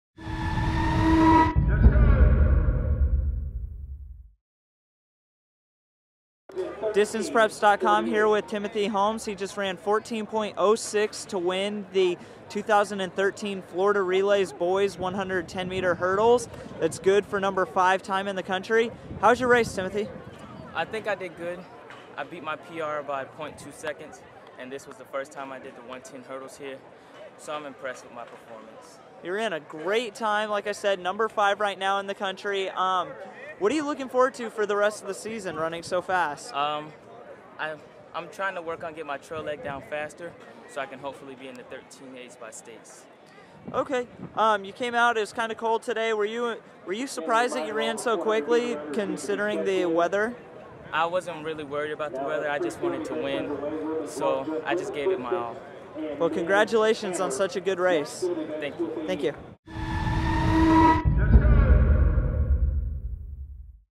Timothy Holmes runs 14.06 for the 110mH win at FLRelays
FLRELAYS